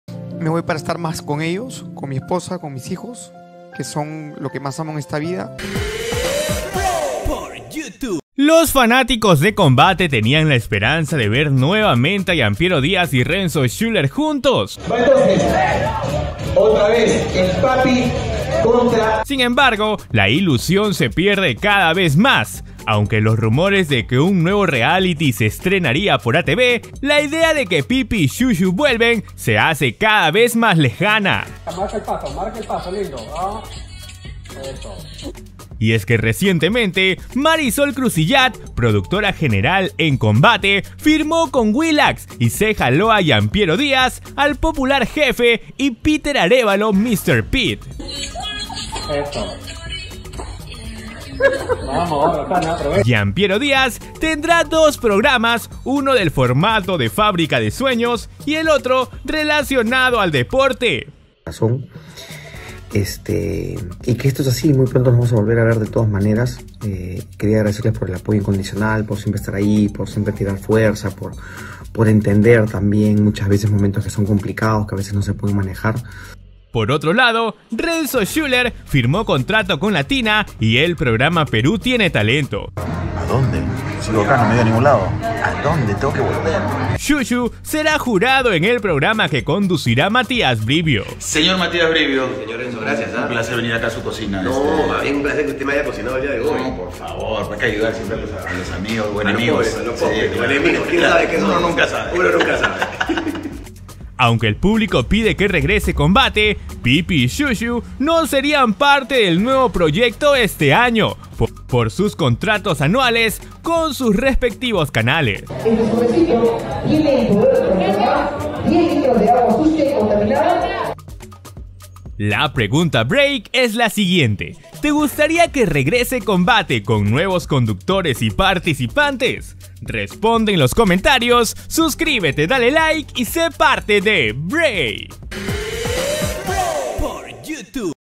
GIAN PIERO DIAZ Y RENZO SCHULLER SE OLVIDAN DE COMBATE 2022 PARA TRISTEZA DE LOS FANS?
Gian Piero Diaz y Renzo Schuller no serian parte de los nuevos proyectos de ATV tras firmar por otros canales.